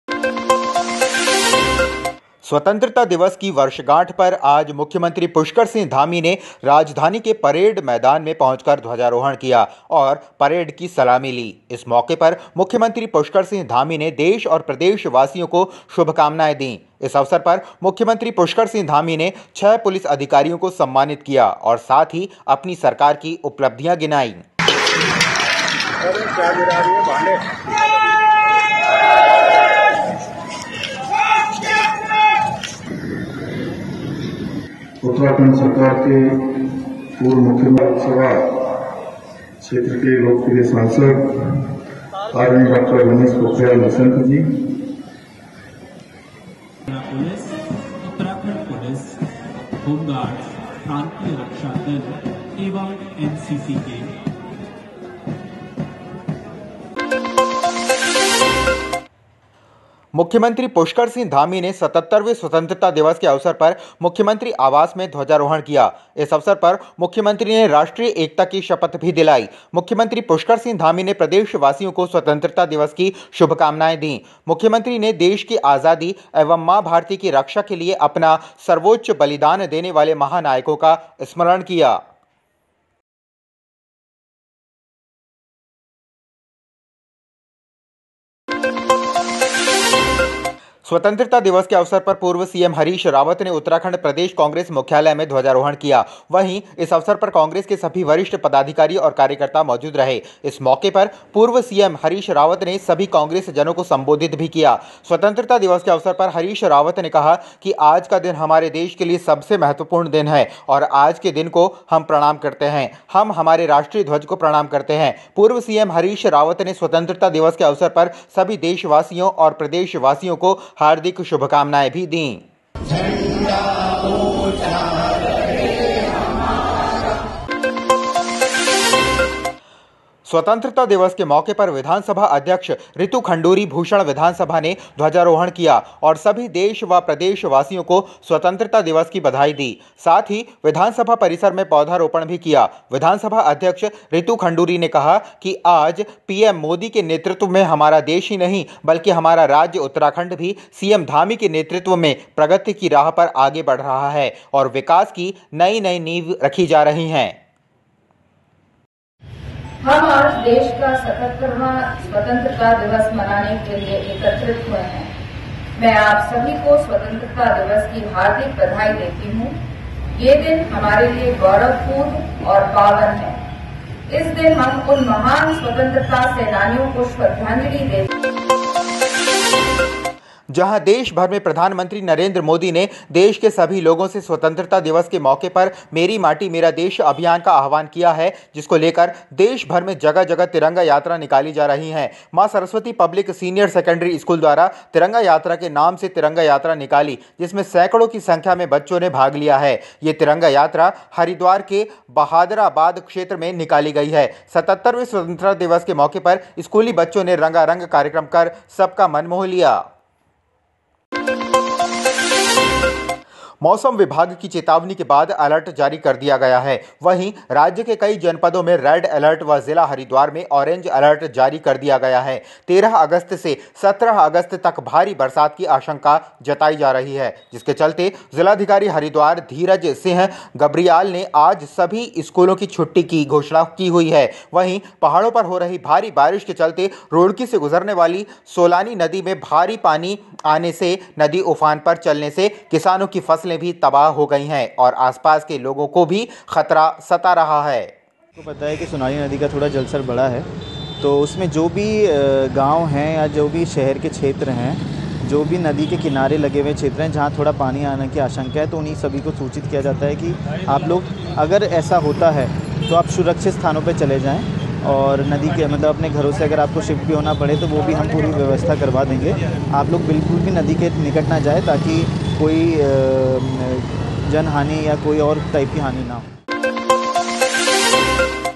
Uttrakhand सीएम ने परेड मैदान में ध्वजारोहण किया । EMS TV 15-Aug-2023
स्वतंत्रता दिवस की वर्षगांठ पर आज मुख्यमंत्री पुष्कर सिंह धामी ने राजधानी के परेड मैदान में पहुंचकर ध्वजारोहण किया और परेड की सलामी ली। इस मौके पर मुख्यमंत्री पुष्कर सिंह धामी ने देश और प्रदेशवासियों को शुभामनाएं दी। इस अवसर पर मुख्यमंत्री पुष्कर सिंह धामी ने 6 पुलिस अधिकारियों को सम्मानित किया और साथ ही अपनी सरकार की उपलब्धियां गिनाई।

मुख्यमंत्री  पुष्कर सिंह धामी ने 77 वें स्वतंत्रता  दिवस के अवसर पर मुख्यमंत्री आवास में ध्वजारोहण किया। इस अवसर पर मुख्यमंत्री ने राष्ट्रीय एकता की शपथ भी दिलाई  मुख्यमंत्री  पुष्कर सिंह धामी ने प्रदेशवासियों को स्वतंत्रता दिवस की शुभकामना  दी। मुख्यमंत्री ने देश की आजादी एवं मां भारती की रक्षा के लिए अपना सर्वोच्च बलिदान देने वाले महानायकों का स्मरण किया।

स्वतंत्रता दिवस के मौके पर विधानसभा अध्यक्ष ऋतु खंडूरी भूषण विधानसभा ने ध्वजारोहण किया और सभी देश व प्रदेशवासियों को स्वतंत्रता दिवस की बधाई दी। साथ ही विधानसभा परिसर में पौधा रोपण भी किया। विधानसभा अध्यक्ष रितु खंडूरी ने कहा कि आज पीएम मोदी के नेतृत्व में हमारा देश ही नहीं बल्कि हमारा राज्य उत्तराखंड भी सीएम धामी के नेतृत्व में प्रगति की राह पर आगे बढ़ रहा है और विकास की नई-नई नींव रखी जा रही है।

 स्वतंत्रता दिवस के अवसर पर पूर्व सीएम हरीश रावत ने उत्तराखंड प्रदेश कांग्रेस मुख्यालय में ध्वजारोहण किया। वहीं इस अवसर पर कांग्रेस के सभी वरिष्ठ पदाधिकारी और कार्यकर्ता मौजूद रहे। इस मौके पर पूर्व सीएम हरीश रावत ने सभी कांग्रेसजनों को संबोधित भी किया। स्वतंत्रता दिवस के अवसर पर हरीश रावत ने कहा कि आज का दिन हमारे देश के लिए सबसे महत्वपूर्ण दिन है और आज के दिन को हम प्रणाम करते हैं हम हमारे राष्ट्रीय ध्वज को प्रणाम करते हैं। पूर्व सीएम हरीश रावत ने स्वतंत्रता दिवस के अवसर पर सभी देशवासियों प्रदेशवासियों को हार्दिक शुभकामनाएं भी दीं।

जहाँ देश भर में प्रधानमंत्री नरेन्द्र मोदी ने देश के सभी लोगो से स्वतंत्रता दिवस के मौक़े पर मेरी माटी मेरा देश अभियान का आवाहन किया है जिसको लेकर देश भर में जगह जगह तिरंगा यात्रा निकाली जा रही है। माँ सरस्वती पब्लिक सिनियर सेकंडरी स्कूल द्वारा तिरंगा यात्रा के नाम से तिरंगा यात्रा निकाली जिसमें सैकड़ो की संख्या में बच्चो ने भाग लिया है। ये तिरंगा यात्रा हरिद्वार के बहादराबाद छेत्र में निकाली गयी है। 77 वे स्वतंत्रता दिवस के मौके पर स्कूली बच्चों ने रंगारंग कार्यक्रम कर सबका मन मोह लिया

मौसम विभाग की चेतावनी के बाद अलर्ट जारी कर दिया गया है वहीं राज्य के कई जनपदों में रेड अलर्ट व जिला हरिद्वार में ऑरेंज अलर्ट जारी किया गया है 13 अगस्त से 17 अगस्त तक भारी बरसात की आशंका जताई जा रही है जिसके चलते जिलाधिकारी हरिद्वार धीराज सिंह गब्रियाल ने आज सभी स्कूलों की छुट्टी की घोषणा की हुई है। वहीं पहाड़ों पर हो भारी बारिश के चलते रुड़की से गुजरने वाली सोलानी नदी में भारी पानी आने से नदी उफान पर चलने से किसानों की फसलें भी तबाह हो गई है और आसपास  के लोगों को भी खतरा सता रहा है।